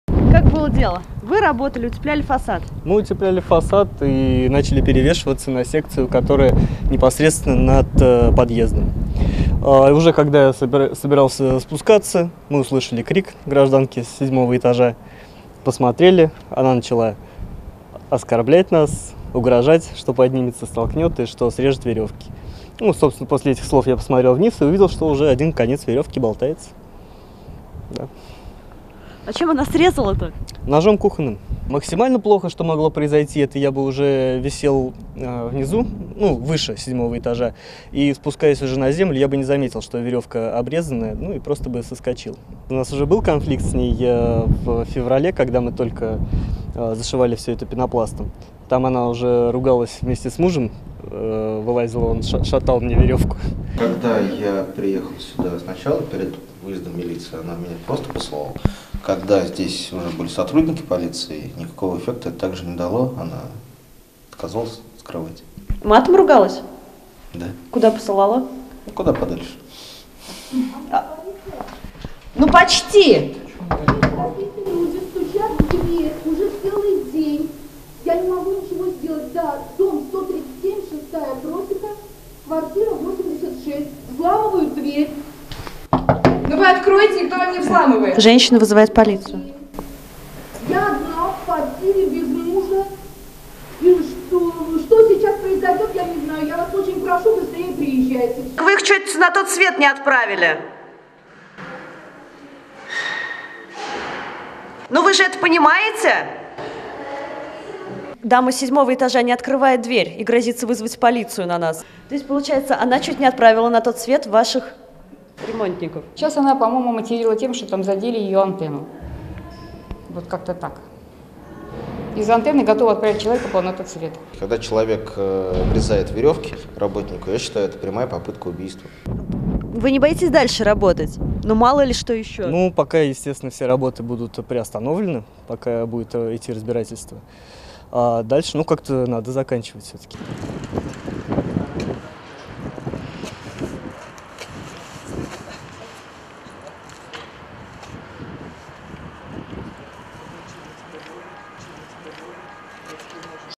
Промышленным альпинистам обрезали трос
Жительница Самары обрезала страховочный трос промышленным альпинистам, выполнявшим ремонтные работы на высоте.